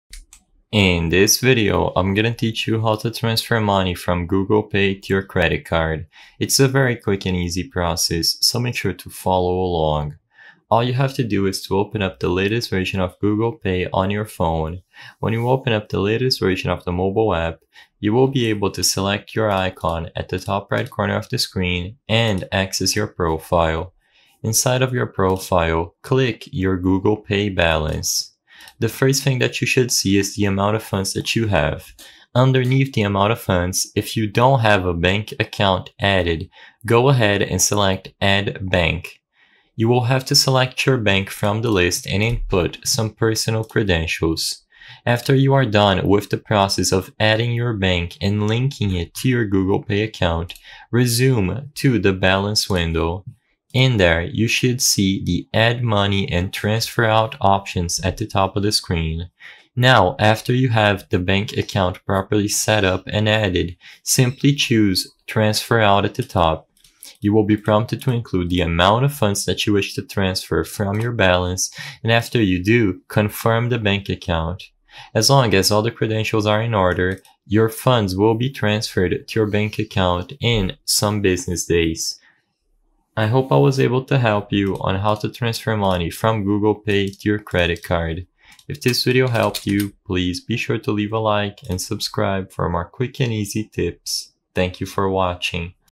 How To Transfer Money from Google Pay to Credit Card (EASY!)
In this video I will solve your doubts about how to transfer money from google pay to credit card, and whether or not it is possible to do this.

I hope I have helped you with the video!